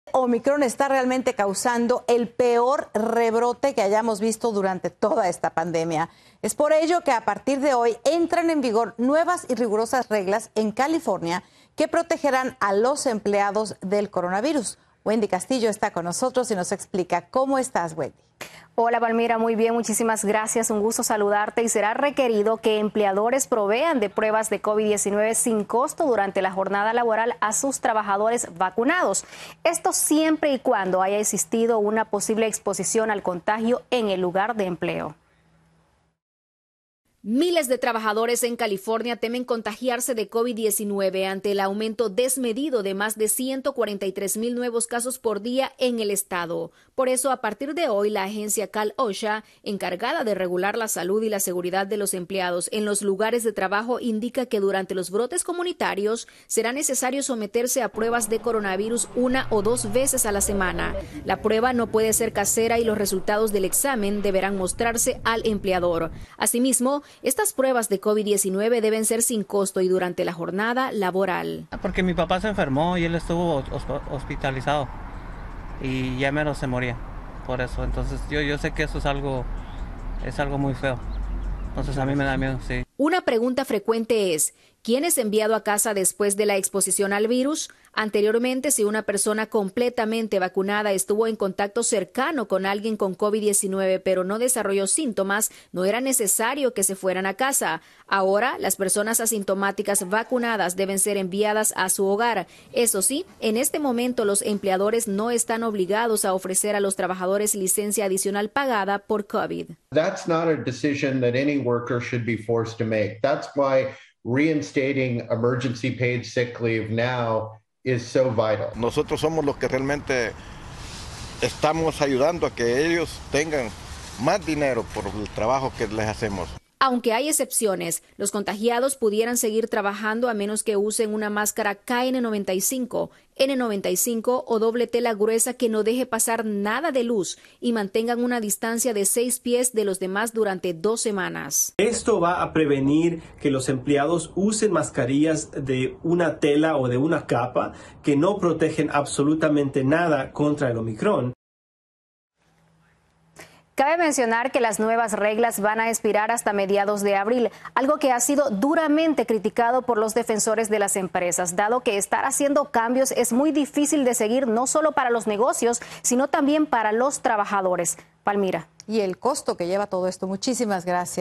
Pruebas de COVID-19 para trabajadores de LA | Noticiero EstrellaTV
Trabajadores de LosÁngeles deberán someterse a pruebas de COVID19 una o dos veces por semana, las cuales serán sin costo y durante la jornada laboral.

Sigue NoticieroEstrellaTV y entérate de las noticias más importantes del mundo y la comunidad hispana en Estados Unidos